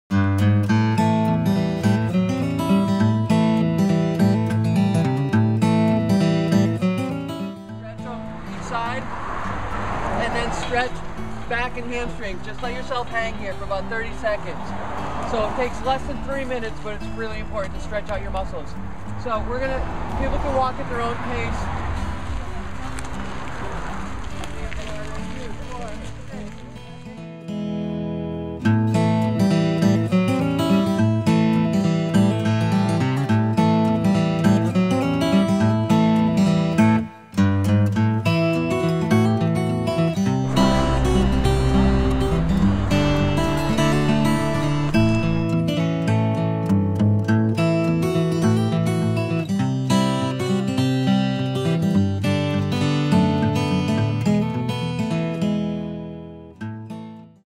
Jog Walk Saturdays With Wendy
Wendy McGannon, personal fitness trainer and owner of McGannon Fitness and Nutrition in Southampton, Massachusetts leads a group of women on a 3.2 mile (5K) Jog/Walk at Whiting Reservoir in Holyoke, MA.  The group is preparing for a 5K Run Walk road race in late April.

The group currently meets at the Whiting Reservoir gate on Rt. 141 at 1:00 PM on Saturdays, beginning Feb. 2, 2013, weather permitting.  There is no cost to join. All are welcome.